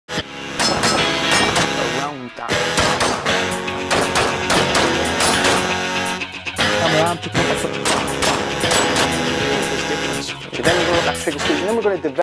Paul Hutchinson YouTube Trailer
Pistol Trailer

A look at Associated Risks Group pistol course led by @paulhutchinsonARG